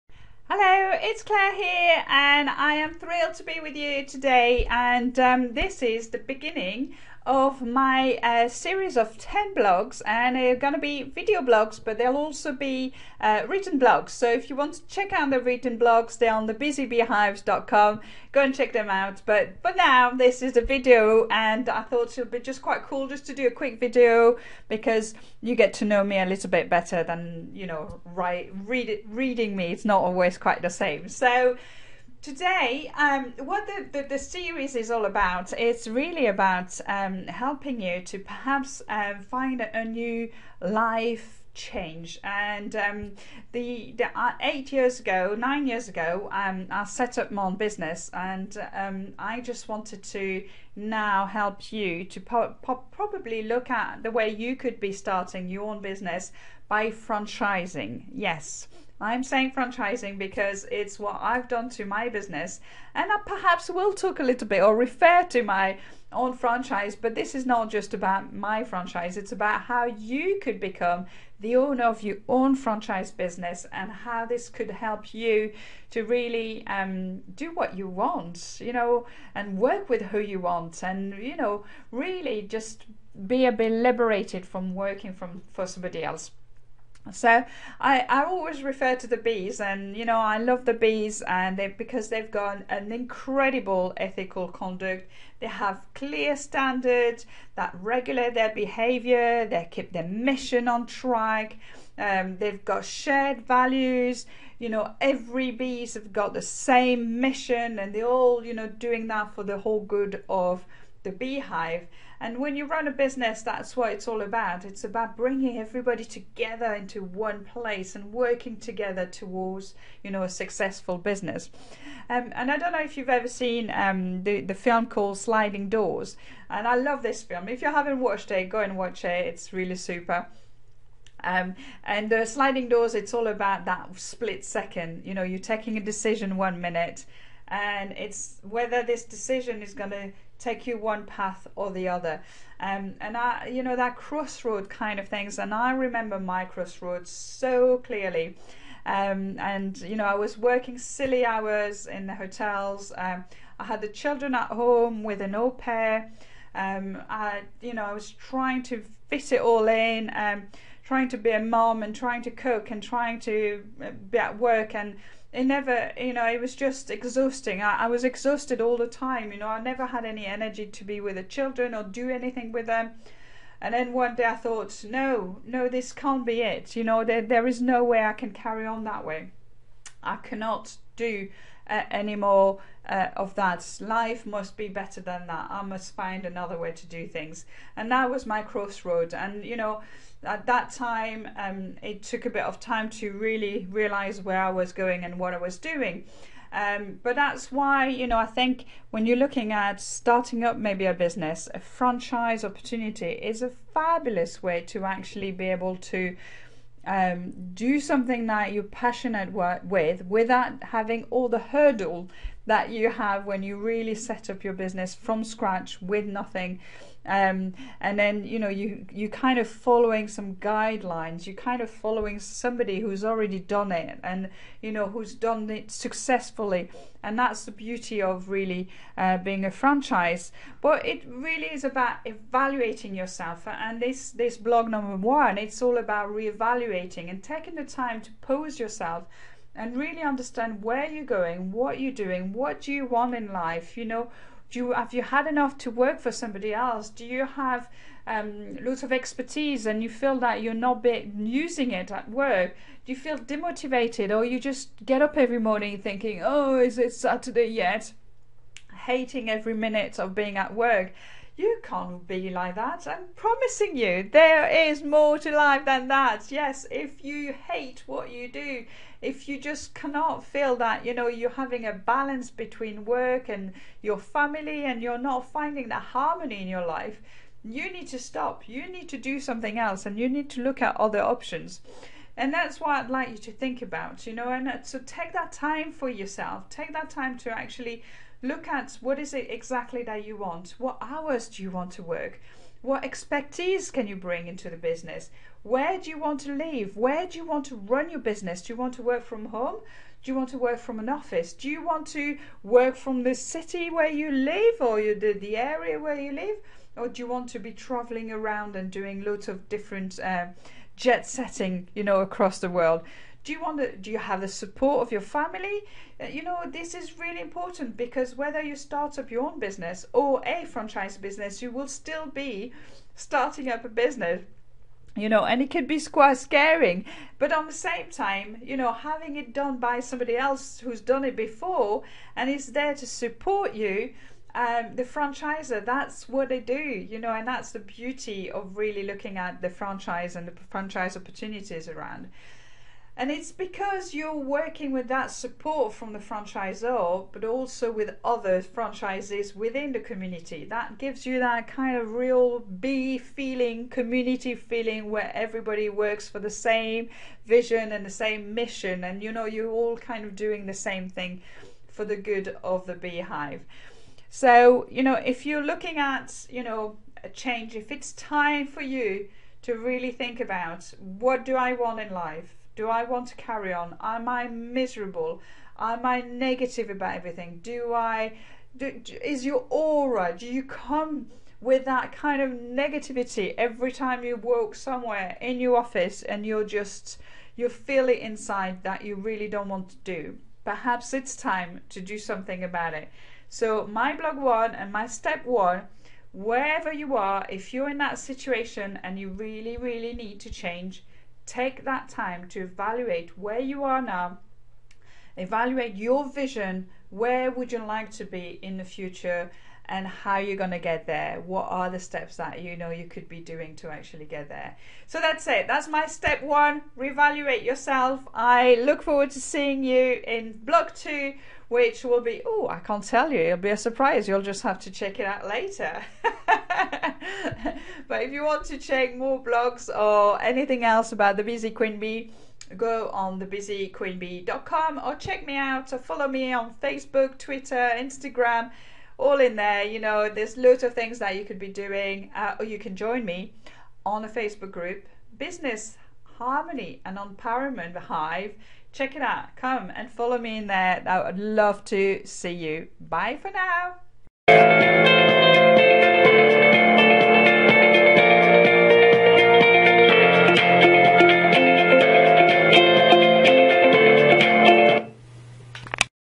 How to… buy the right Franchise Business for you - Blog 1 Evaluate yourself
Join me for the first of my video blog series "How to... buy the right Franchise for you" and being your own boss.

Talking to you about crossroad and how it come to a time in our life where we want more than just a 9-5 job...

You can also check out my Mystery Shopping and Customer Experience Franchise opportunity helping your local businesses delivering exceptional customer service.